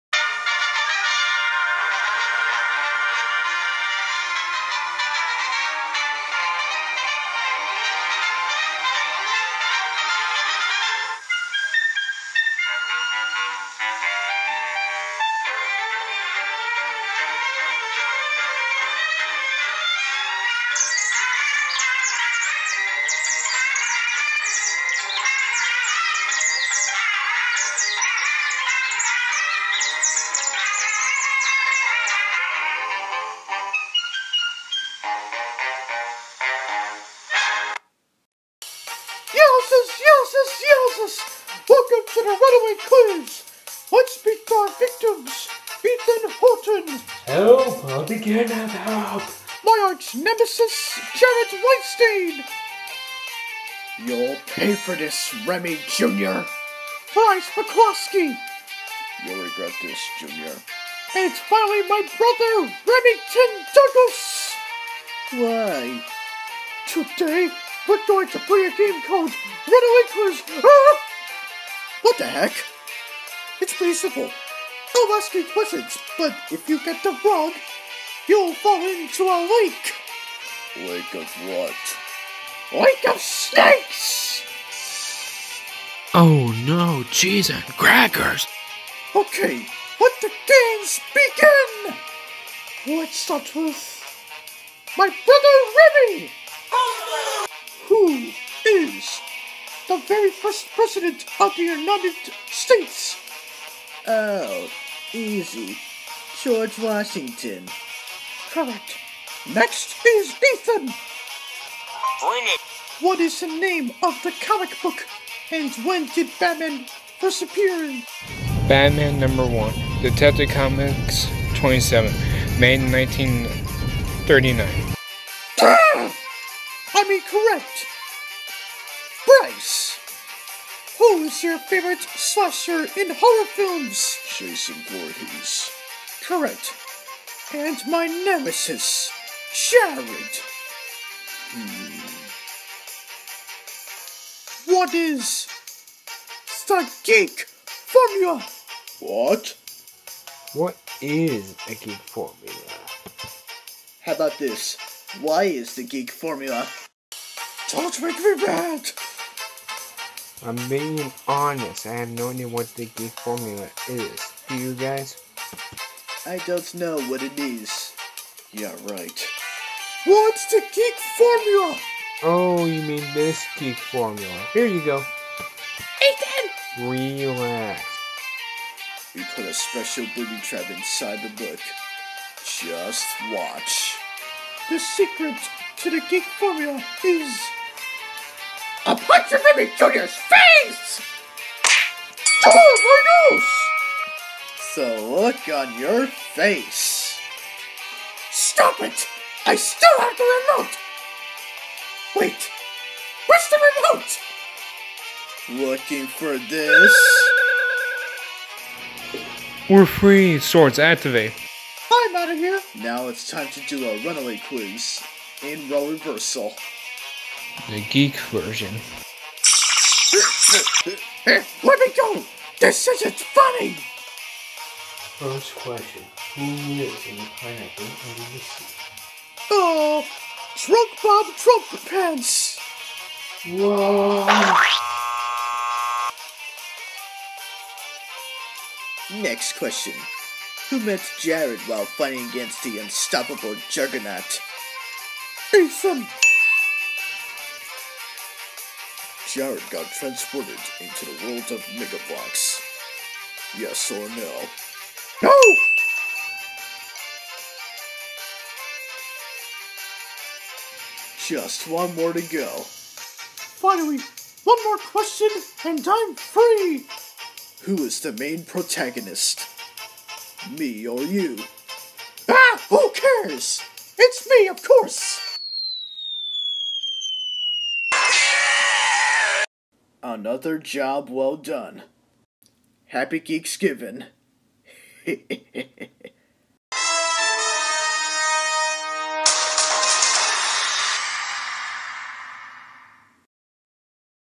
Geek Studios Shorts: Remi Jr.’s Runaway Quiz
Remi Jr. has kidnapped the Geek Studios in the game show.  Can they win or lose?